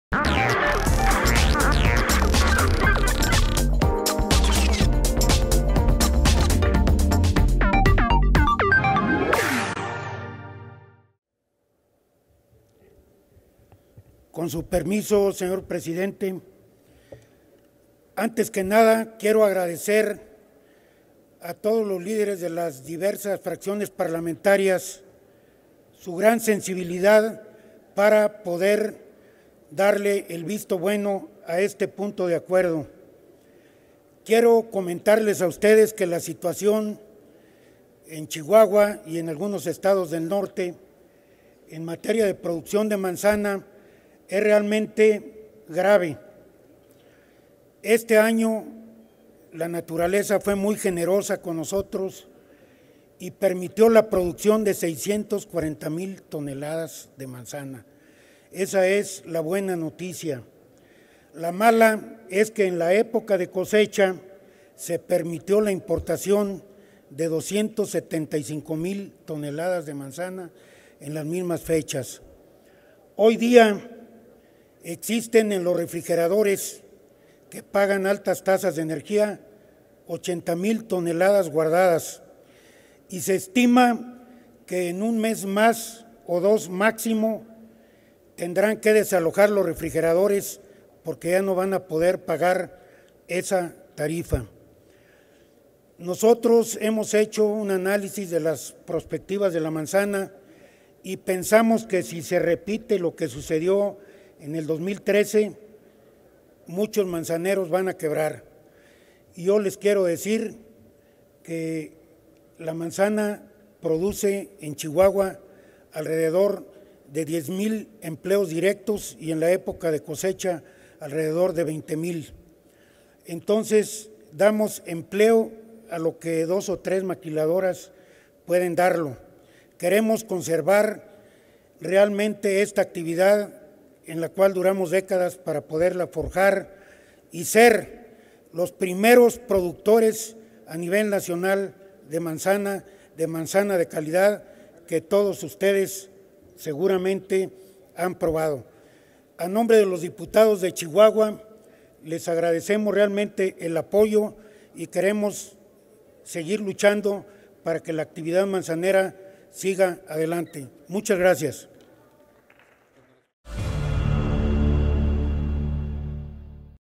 Dip. Kamel Athie (PRI) - Regular Cuotas de Importación y Exportación de la Manzana
El Dip. Kamel Athie Flores (PRI) reconoce la sensibilidad para presentar el acuerdo a discusión, sin embargo, cuestiona la importación de 275 mil toneladas de manzanas, sostiene que en Chihuahua se generan 10 mil empleos directos y en época de cosechas cerca de 20 mil. Solicita el apoyo al presente acuerdo.